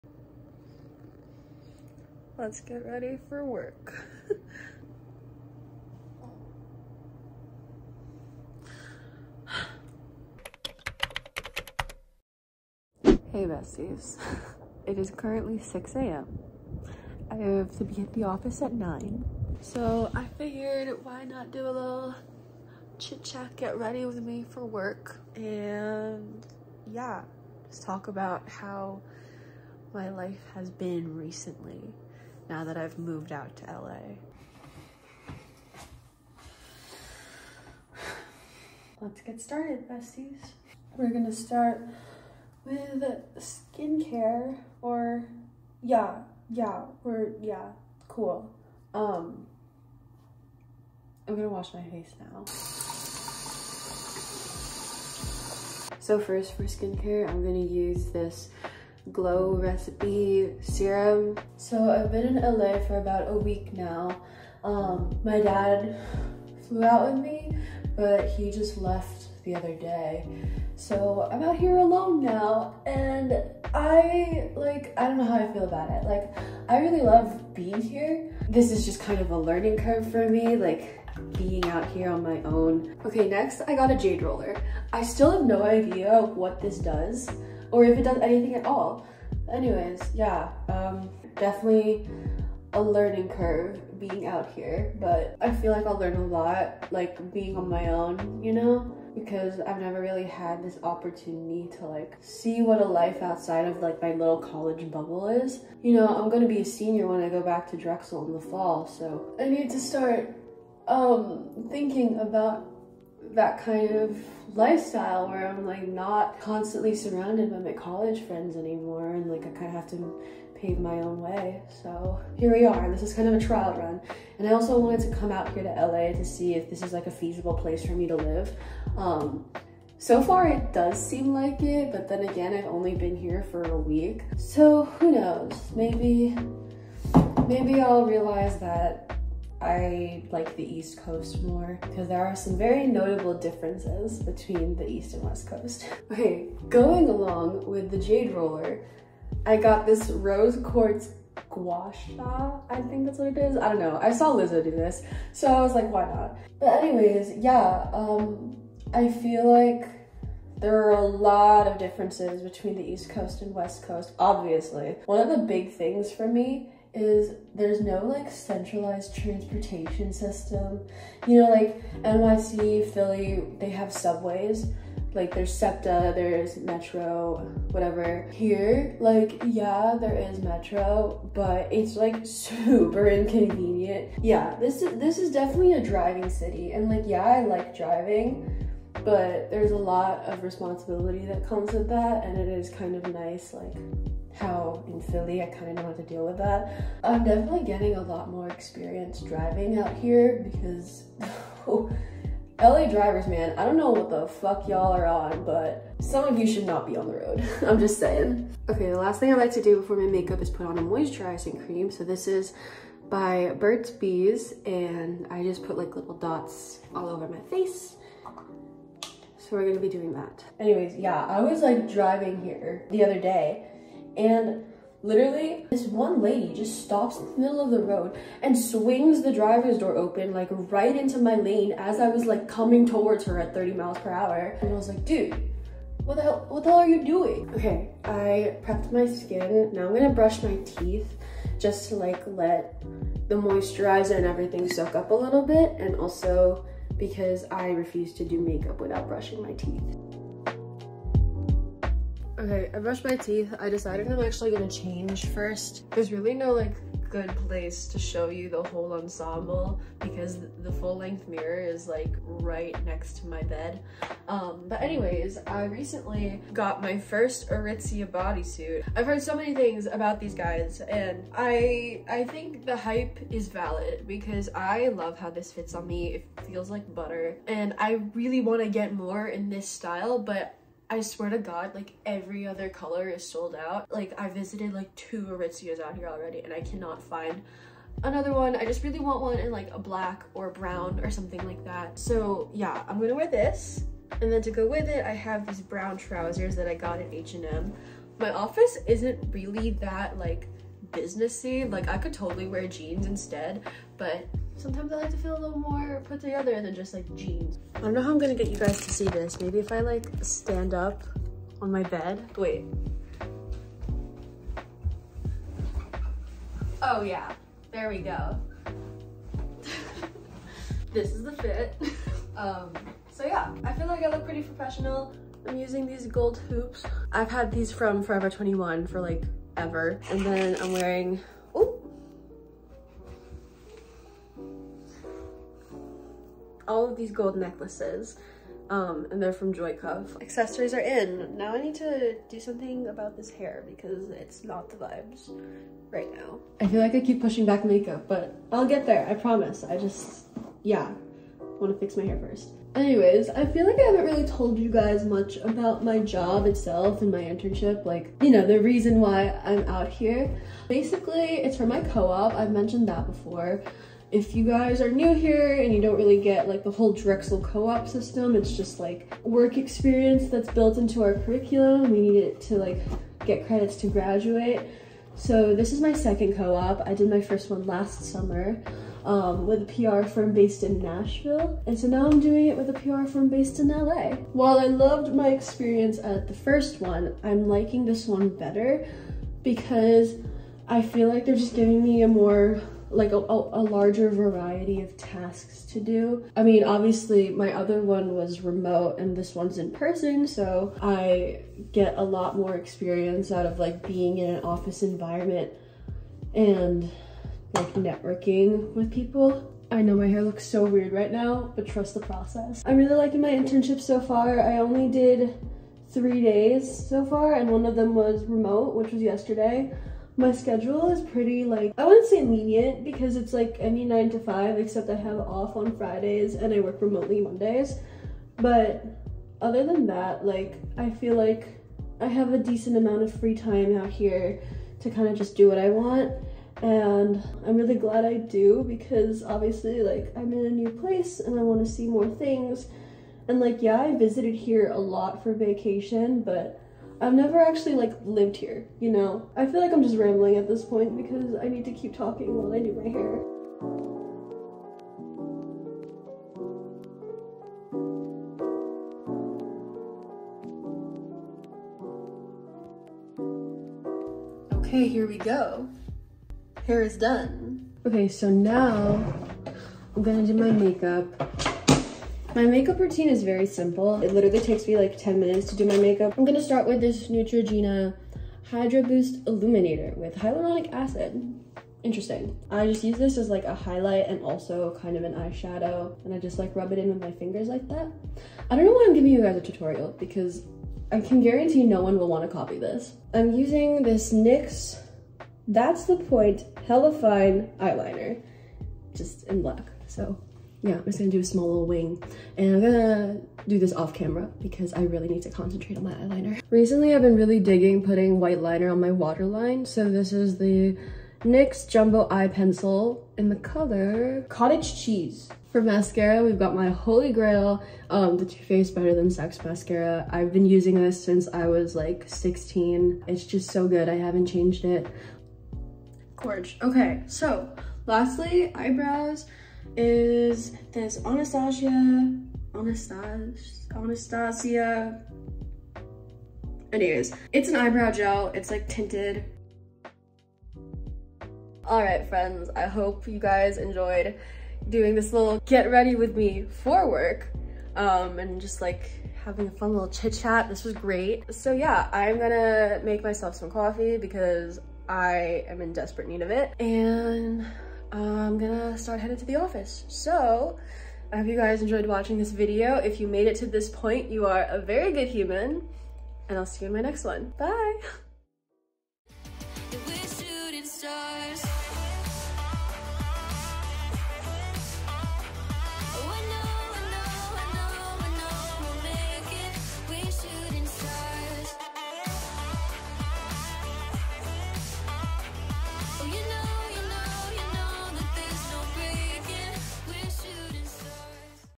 chit chat grwm for the office! // Ashley Claire
hi besties :)
hope you enjoyed watching me get ready and chat with yall as much as I enjoyed filming it. i wanted to give you guys a little insight into what my morning routine is like and also talk a little bit about how i've been doing out here in LA!

More video to come soon...